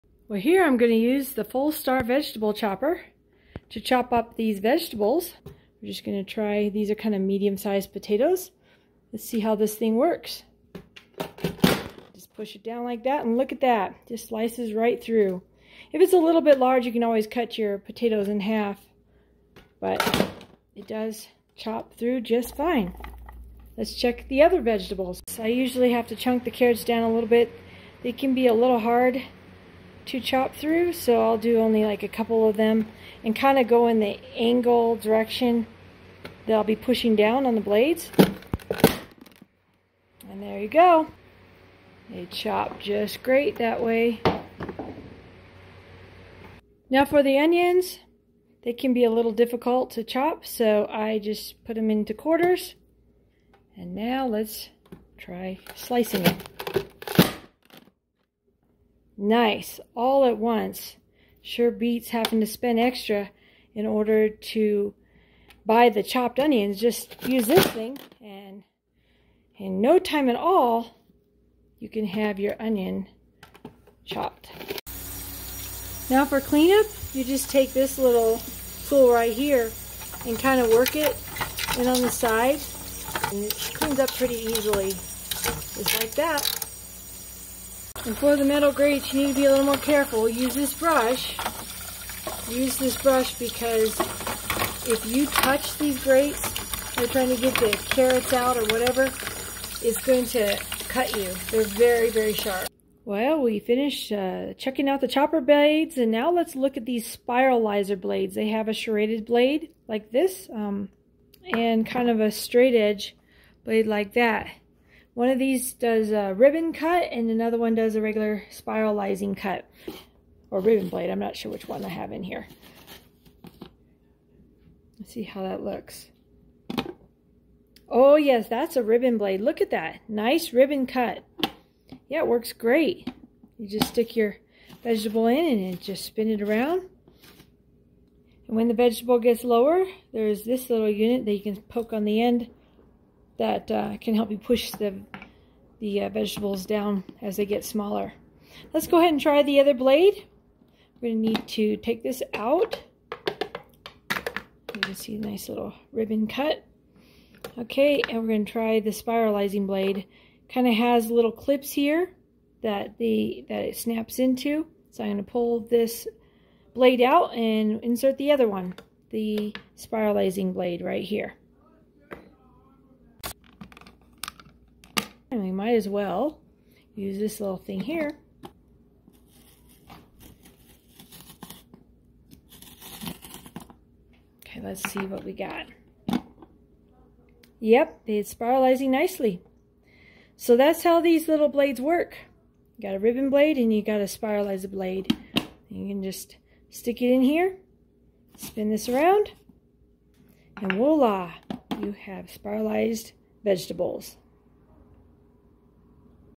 Fullstar All-in-1 Vegetable Chopper Review
This vegetable chopper is easy to use and chops food up very quickly.  I love it!

Quick Link to the Product:

Or View in Christy's Amazon Store: